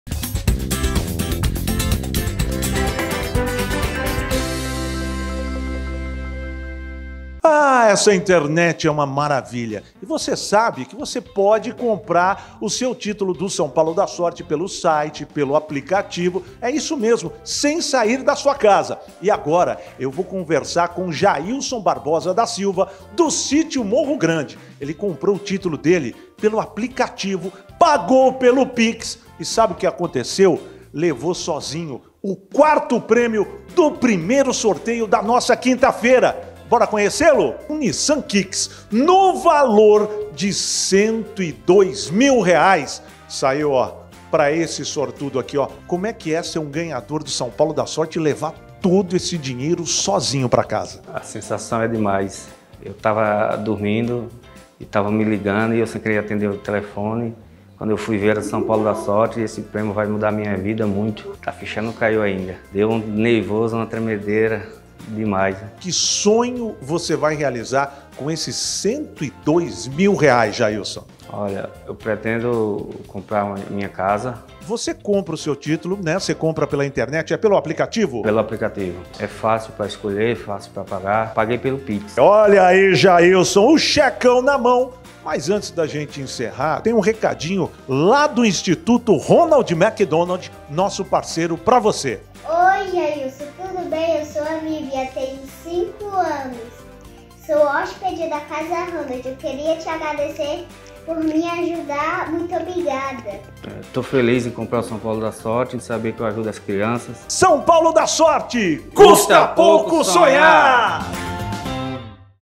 ENTREGA DO 4º PRÊMIO, SÃO PAULO DÁ SORTE DIA 27.04.2023
Confira o sortudo da semana passada (20/04) no São Paulo dá Sorte e faça como ele, garanta já o seu título! Quem sabe você não é um dos próximos ganhadores!?

Nos acompanhe também no TikTok: spdasorte

São Paulo dá Sorte, Custa Pouco Sonhar!